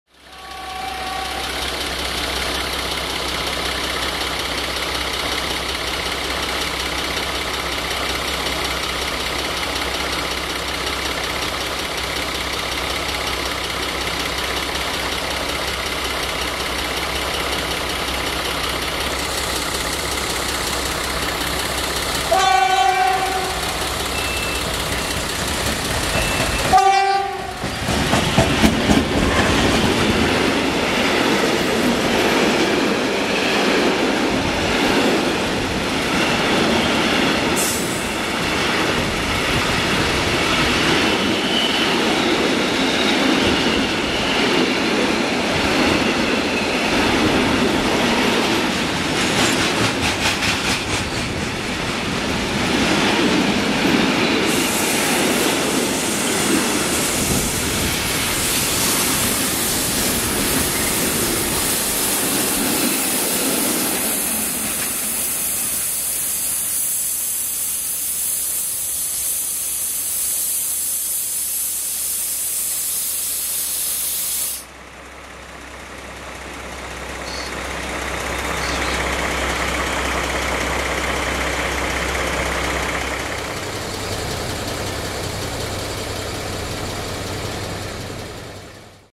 BR232 - Ludmilla With DB Schenker Freight Train in Vadu Crisului Station
BR232 - Ludmilla cu Marfar CD Schanker in Vadu Crisului

1:DB Schenker Freight Train - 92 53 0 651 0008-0
2:Video Ending

© Train97